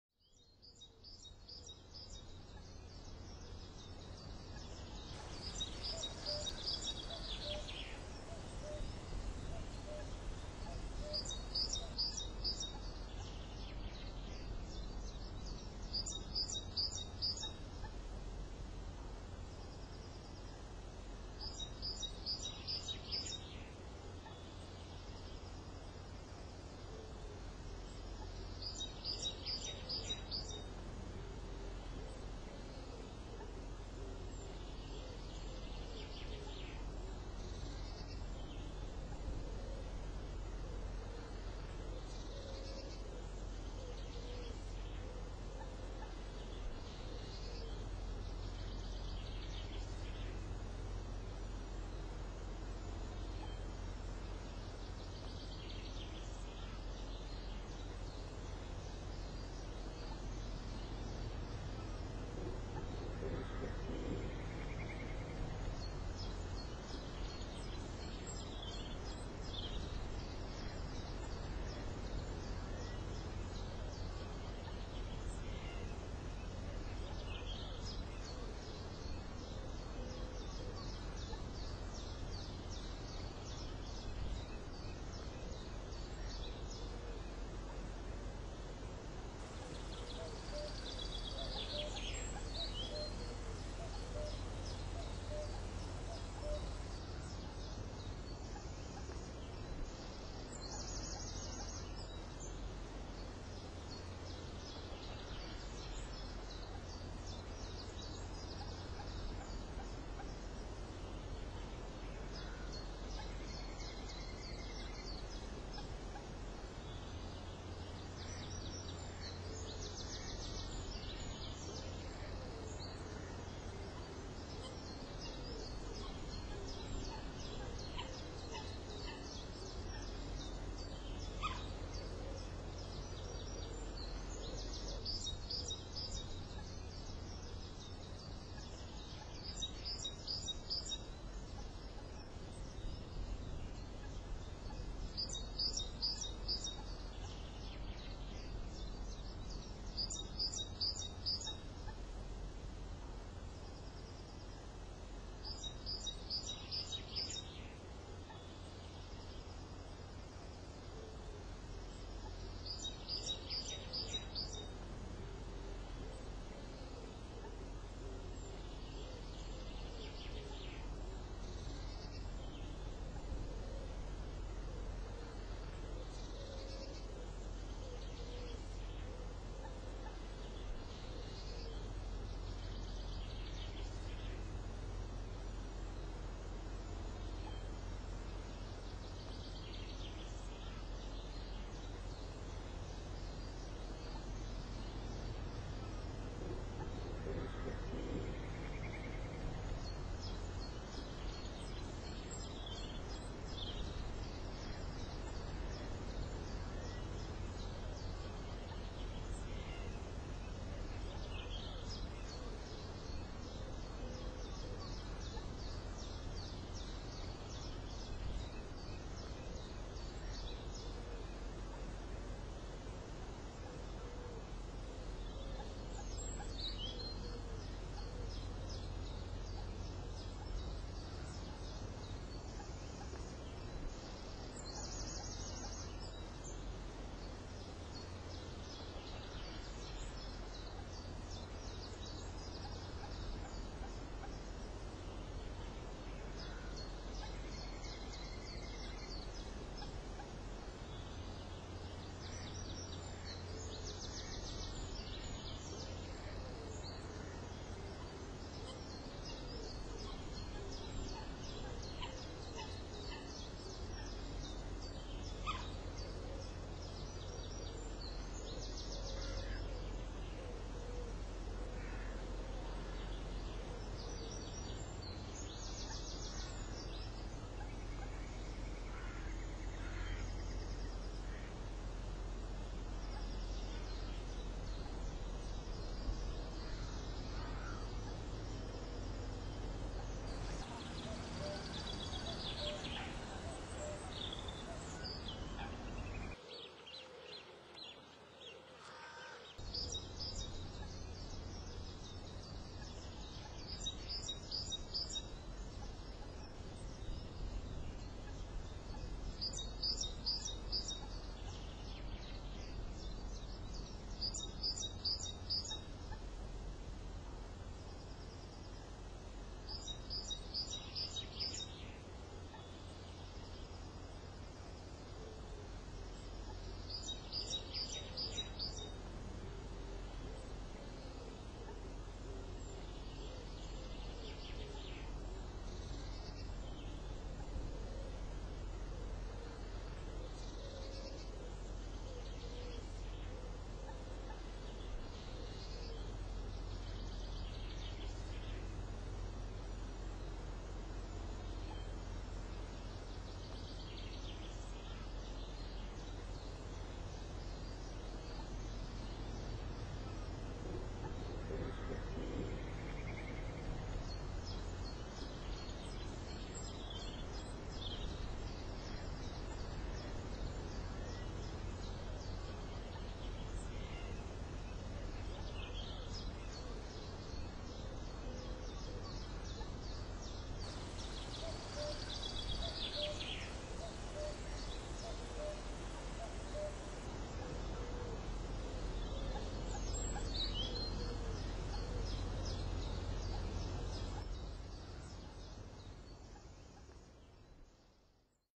WESTHAY ON JUNE THE 30th 2015
It seems there are 3 chicks of the 5 the Great Crested Grebes Eggs layed,now they are hatched the parents keep them a long way off the other side of the water,there is an Otter passing through as well.